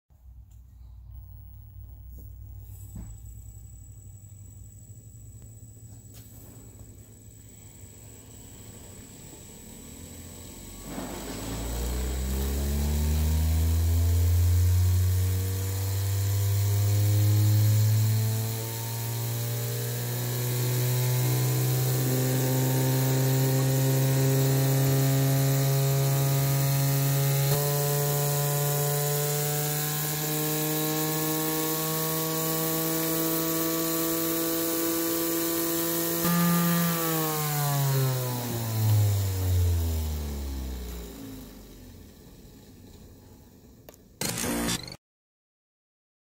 3d Printed Brushless Motor
I pushed this thing to hit 10, 000 rpm without it exploding!